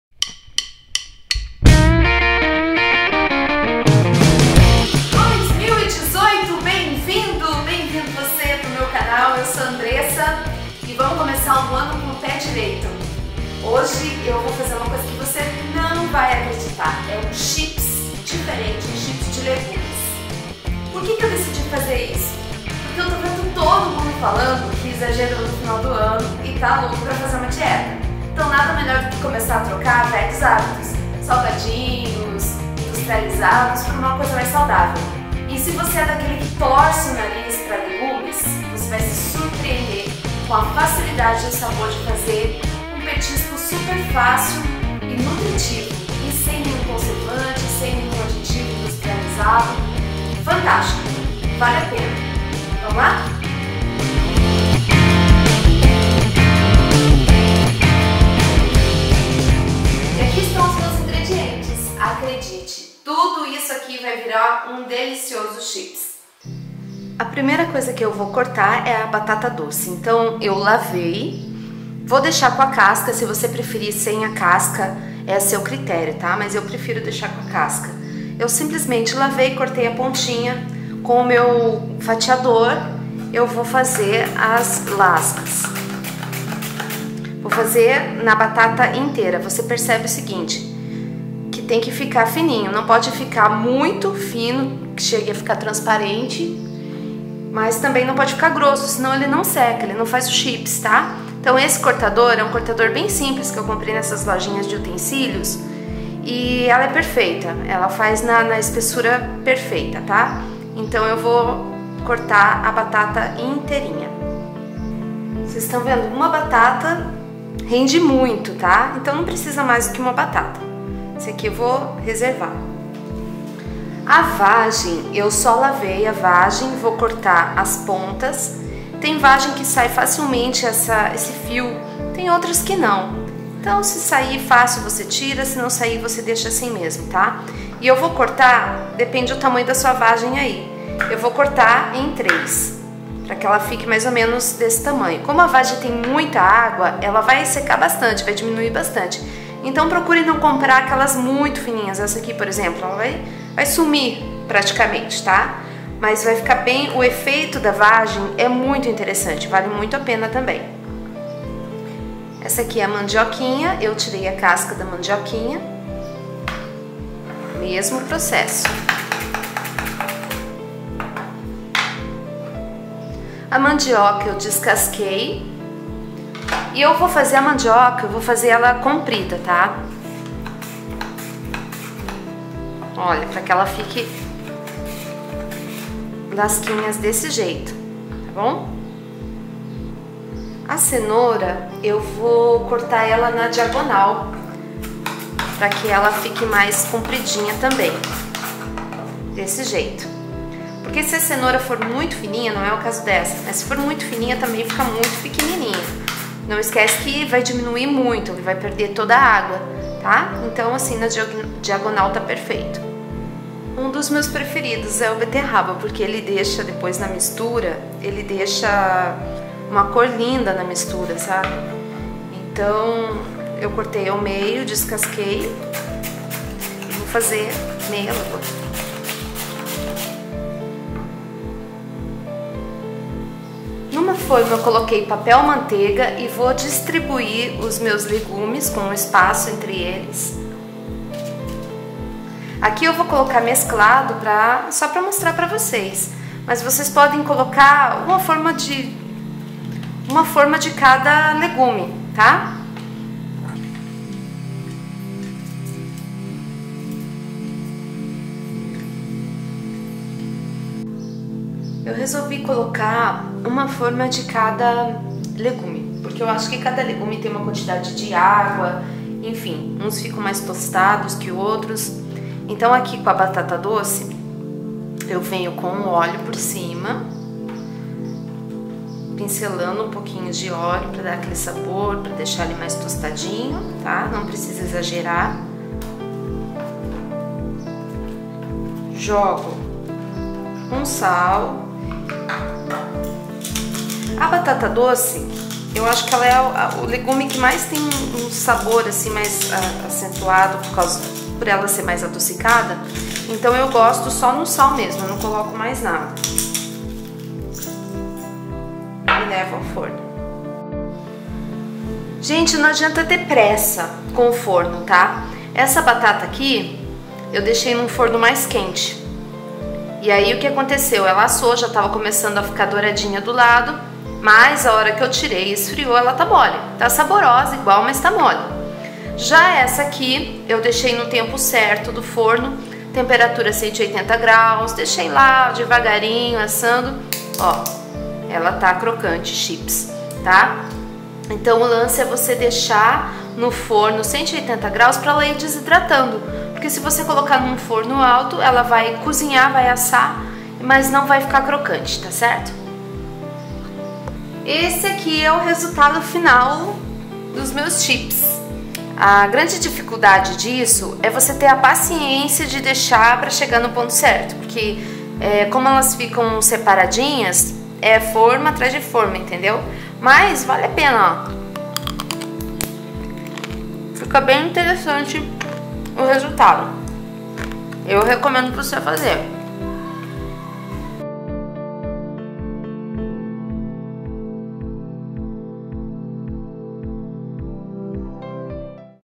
chips de legumes
Nada melhor do que você ter uma opção mais saudável para seus filhos ou pra sua dieta.
 ingredientes:
-1 batata doce
-1 mandioquinha
-1 beterraba
-1 cenoura
-1 punhado de vagem
-1 mandioca

pique os legumes, leve ao forno por aprox. 50 min a 180 graus, numa forma untada e forrada com papel manteiga.

pincele azeite e sal antes de assar.
esta pronto!